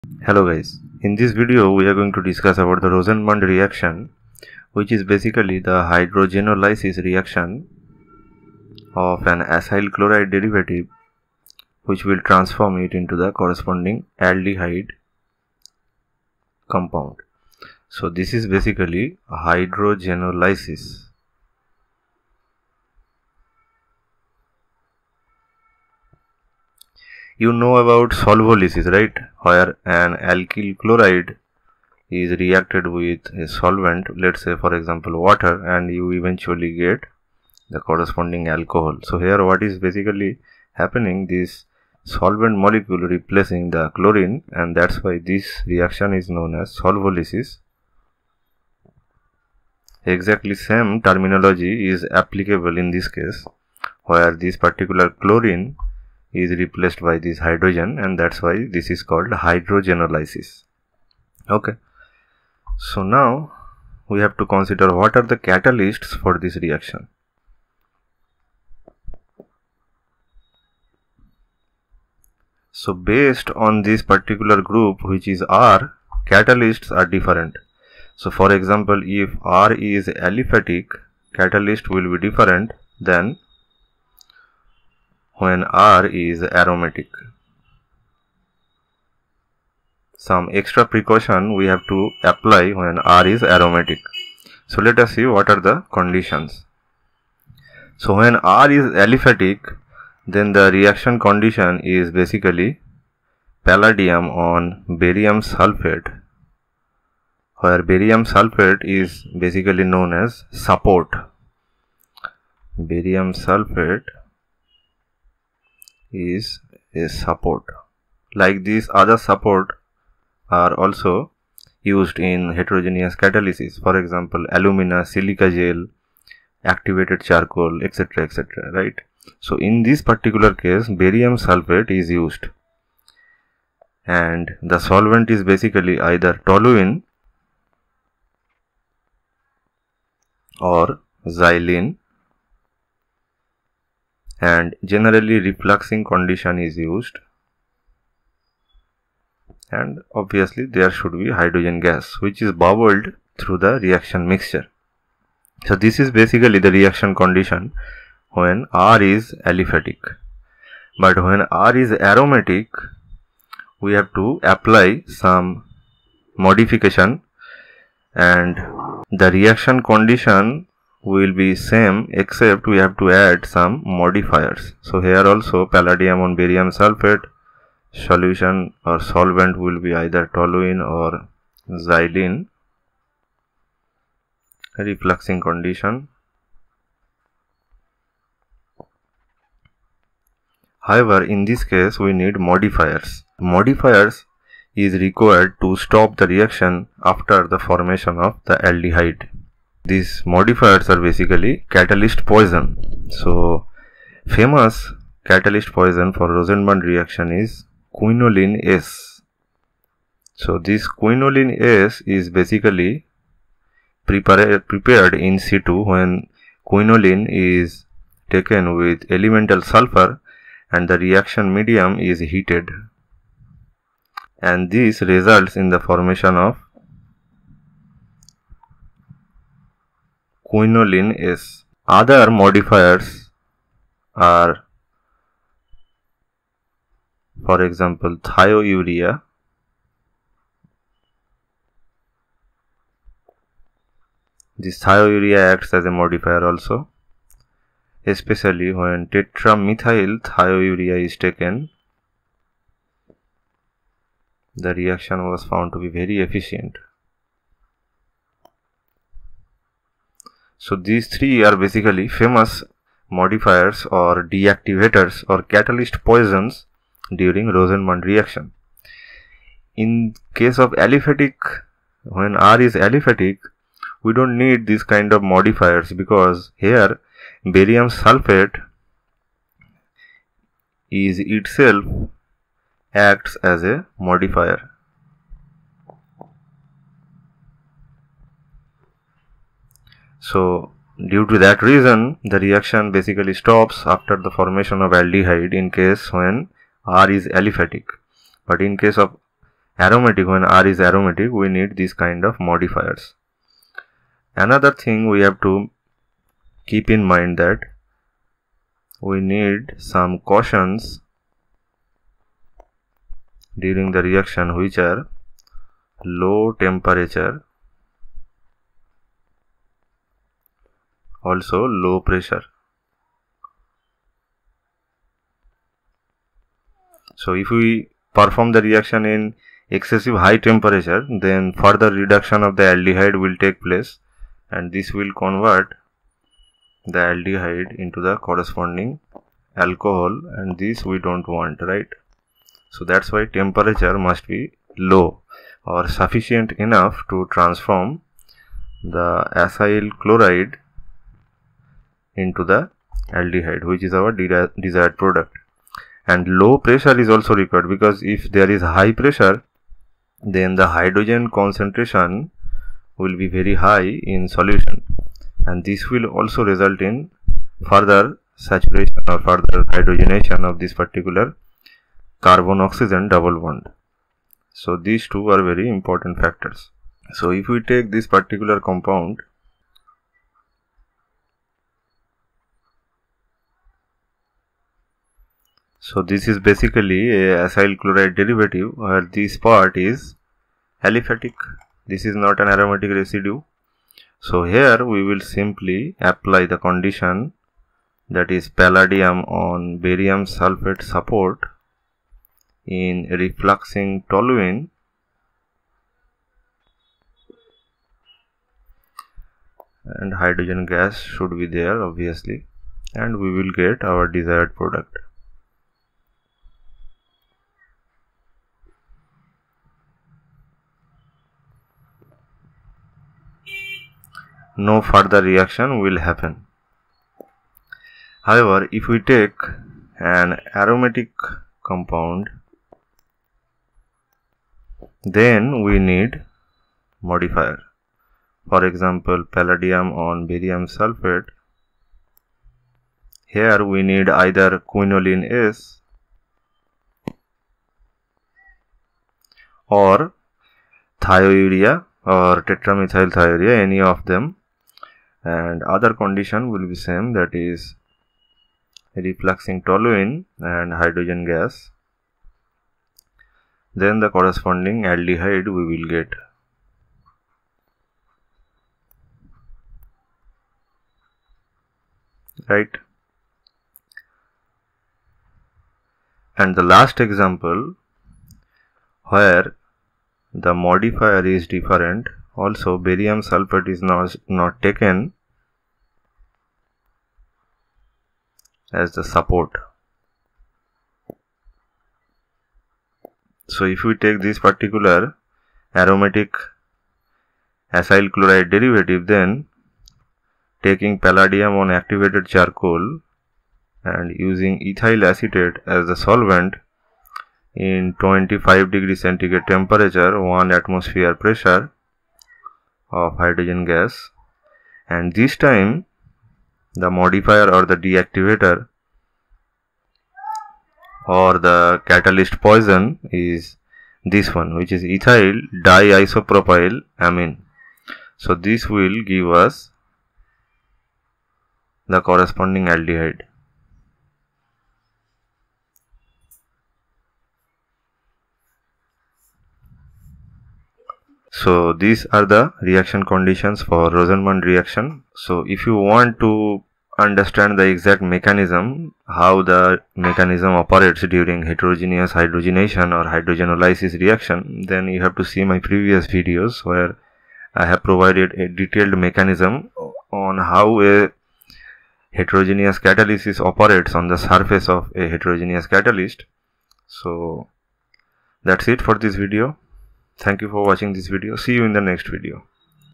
Rosenmund reduction or reaction | Catalytic hydrogenation of Carboxylic acid chlorides | Examples
In this video several problems and solutions on Catalytic hydrogenation of acid chlorides are discussed. It is known as Rosenmund reduction or Rosenmund reaction. Aliphatic and aromatic carboxylic acid chlorides are taken as examples. Palladium on barium sulphate in presence of quinoline-S. are best for this purpose. It will be very helpful for competitive exams like NET and GATE.
Books consulted:
1. Catalytic Hydrogenation: Techniques and Applications in Organic Synthesis by Robert L. Augustine
2. Modern Methods of Organic Synthesis by W. Carruthers